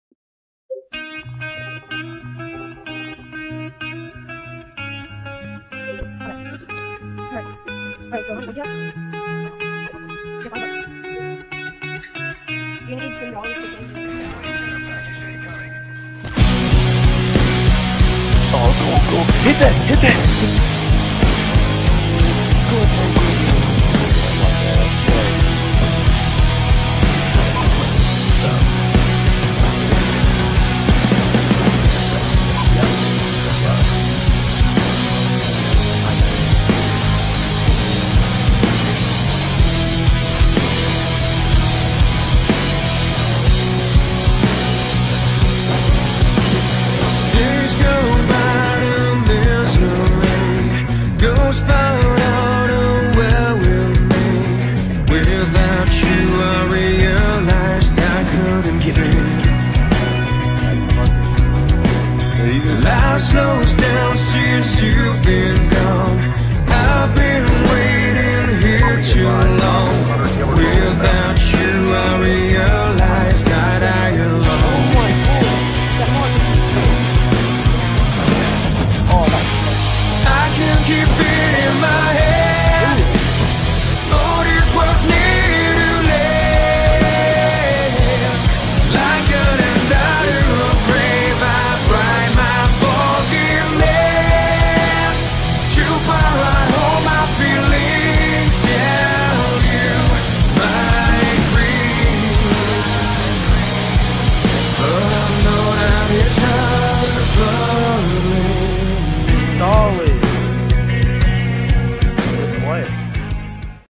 Joined Wave Daytage!
2nd day with the tag because the first was just god awful! I know its not really all that good either but thats because that multi ended my 2 week dry streak! I actually did break my headset on the first clip too xD